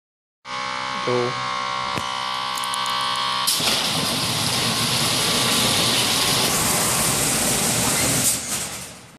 Two Ball Driving Test Aerial Assist
We tested our ability to drive with two balls today. What could this be for?